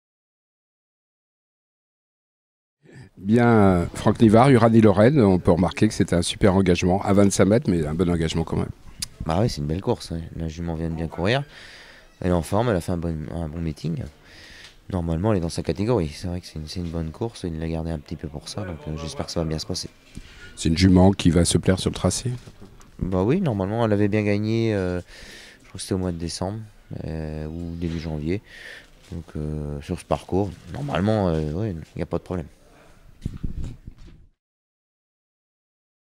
Franck Nivard, driver du 18, Uranie Lorraine, lors du Prix des Vosges dimanche 8 février à Vincennes
Préparez au mieux Prix des Vosges, la 7e course du dimanche 8 février à Vincennes.